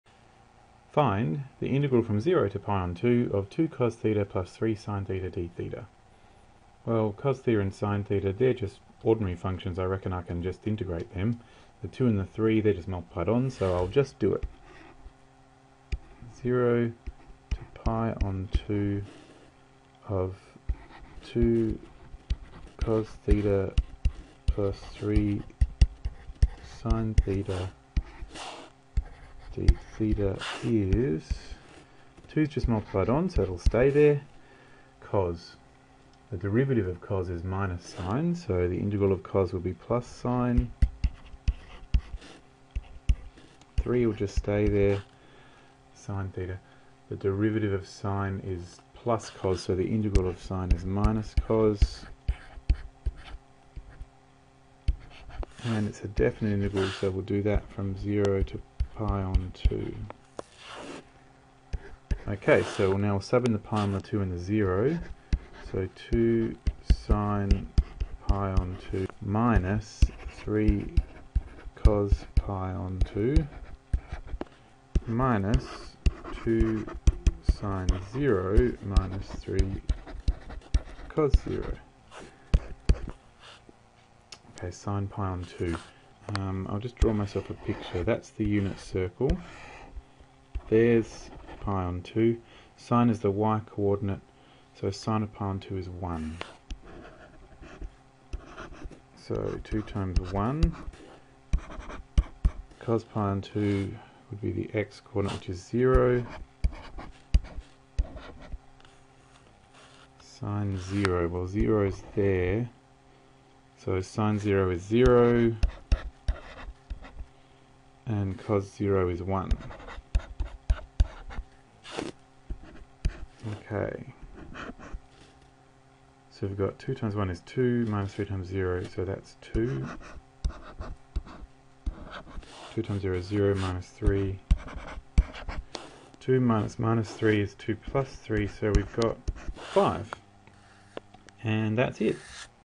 EXAMPLE: Integrating basic trigonometric functions
Worked example by David Butler. Features finding the definite integral of a function constructed from sin and cos.